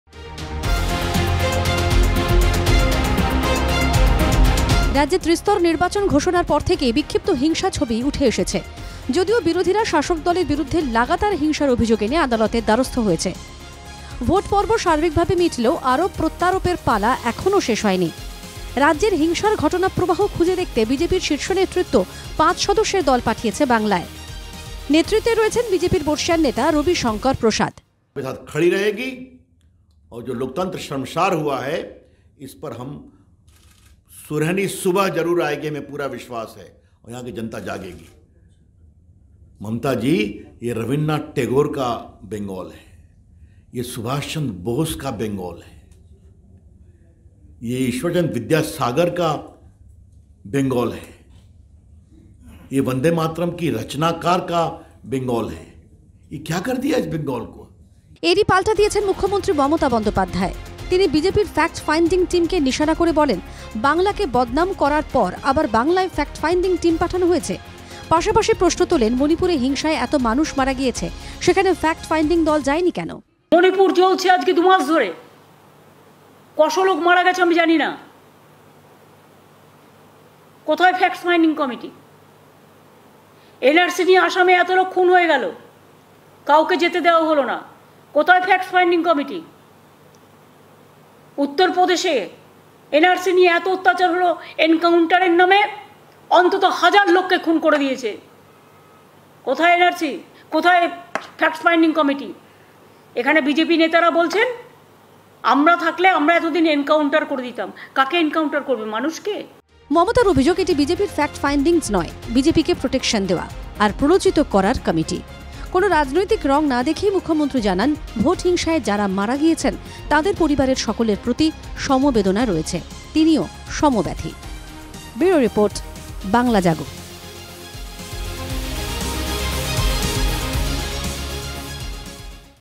Rabishankar: বিজেপিকে তুলোধনা মমতার
ভোটে হিংসার অভিযোগ খতিয়ে দেখতে, বিজেপির কেন্দ্রীয় নেতৃত্ব ফ্যাক্ট ফাইন্ডিং কমিটি পাঠিয়েছে বাংলায়। সেই কমিটিকেই তুলোধনা করলেন মমতা বন্দ্যোপাধ্যায়। তিনি বললেন, বাংলাকে বদনাম করে আবার ফ্ল্যাক্ট ফাইন্ডিং কমিটি! পাশাপাশি মুখ্যমন্ত্রী জানান, ভোটে বিক্ষিপ্ত হিংসায় কয়েকজন মারা গিয়েছে, তাঁদের জন্য দুঃখিত।